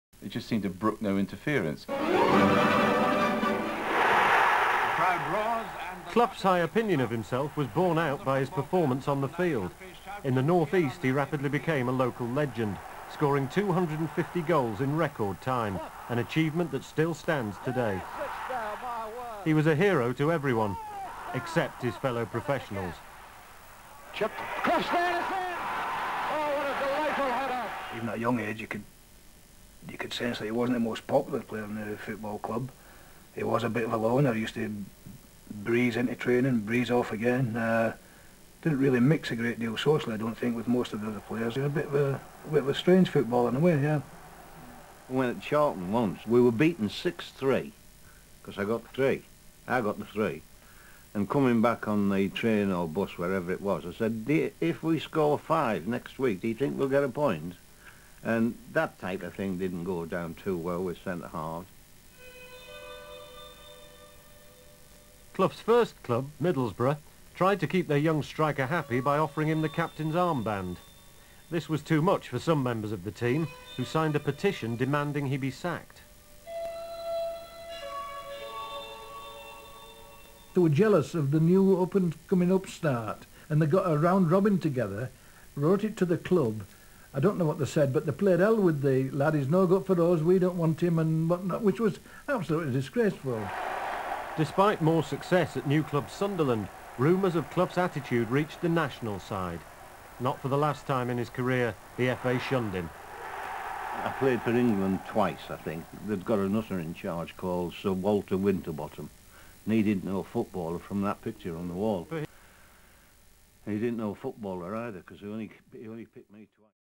BRIAN CLOUGH - ON PLAYING DAYS + SIR WALTER WINTERBOTTOM
Footage of Brian Clough during his playing days, and his relationship with his teammates and what he thought of Sir Walter Winterbottom.